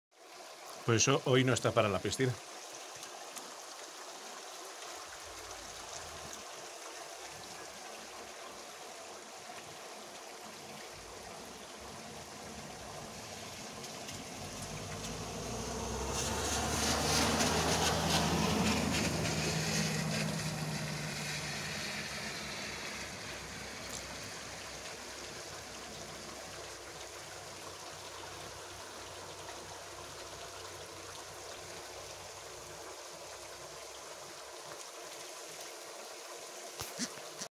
Alerta por lluvias intensas y granizadas
Informativo local
ESTE MISMO MEDIODÍA (sábado 29 de junio) en un pueblo de la comarca de La Bañez